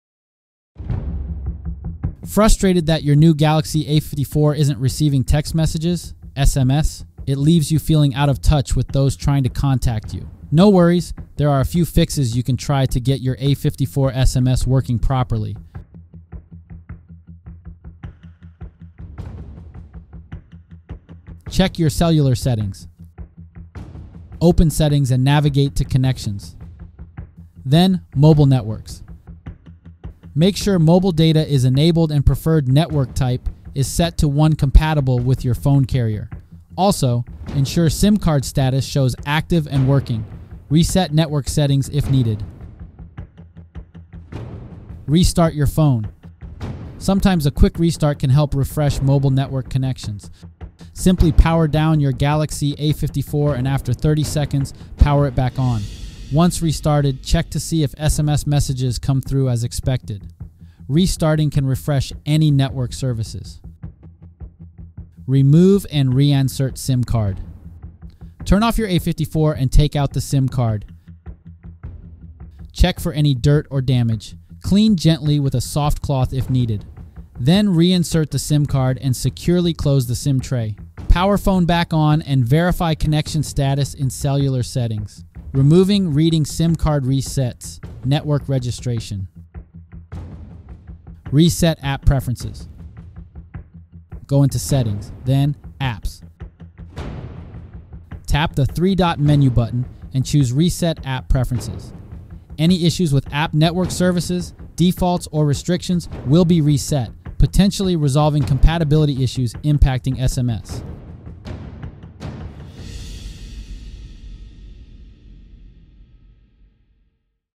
How To Fix Samsung A54 SMS Not Working | Can't Receive Text Messages
Is your new Samsung Galaxy A54 not receiving text messages (SMS)? Don't worry, this video shows you simple fixes to get your A54's SMS working again. We'll guide you through checking your cellular settings, restarting the phone, removing and reinserting the SIM card, and resetting app preferences.